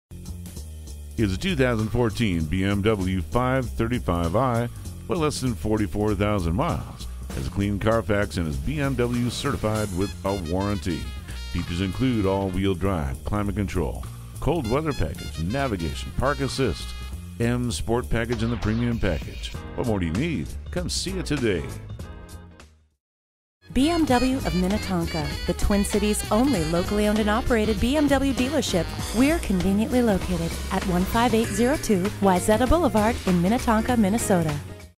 Used 2014 BMW 5 Series Minnetonka MN Minneapolis, MN P51371 - SOLD
Year: 2014
Make: BMW
Model: 5 Series
Trim: 4dr Sdn 535i xDrive AWD 3.0L I6
Engine: 3.0 L I6 Cyl.
Transmission: 8-Speed A/T
Color: Carbon Black Metallic
Interior: Black/Excl. Stitch
Mileage: 43910
Stock #: P51371
VIN: WBA5B3C59ED531848
**BMW CERTIFIED**, **CLEAN CARFAX, **M SPORT PACKAGE, **NAVIGATION, **HEATED SEATS, **BACKUP CAMERA, **PREMIUM PACKAGE, and **COLD WEATHER PACKAGE. brCold Weather Package (Heated Front Seats, Heated Rear Seats, Heated Steering Wheel, and Retractable Headlight Washers), M Sport Package, Premium Package (Comfort Access Keyless Entry, Power Tailgate, and Satellite Radio), BMW Certified, AWD, Black w/Dakota Leather Upholstery, Aerodynamic Kit, Aluminum Hexagon Interior Trim, Anthracite Headliner, LED Fog Lights, M Sport, M Steering Wheel, Navigation System, Shadowline Exterior Trim, and Without Lines Designation Outside. BMW Certified Pre-Owned means you not only get the reassurance of up to a 6yr/100,000 mile limited warranty, but also a multipoint inspection/reconditioning, 24/7 roadside assistance, trip-interruption services, and a vehicle history report.

Address:
15802 Wayzata Blvd